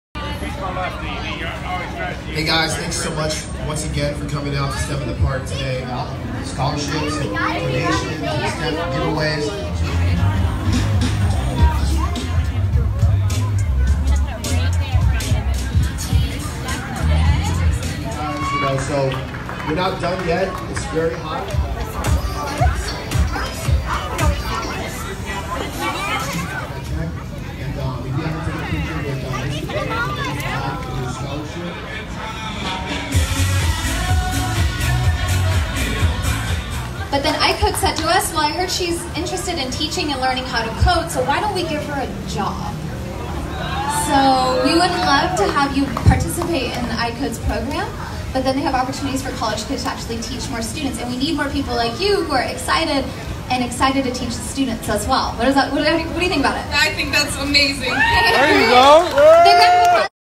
STEM in the Park
What an amazing morning. STEM in the Park was a success and we had a blast celebrating the Science, Technology, Engineering and Mathematics Initiatives that inspire the next generation of inventors, innovators and mobility makers.

Community Content by:
North Texas Ford Dealers
Ford Motor Company
GTB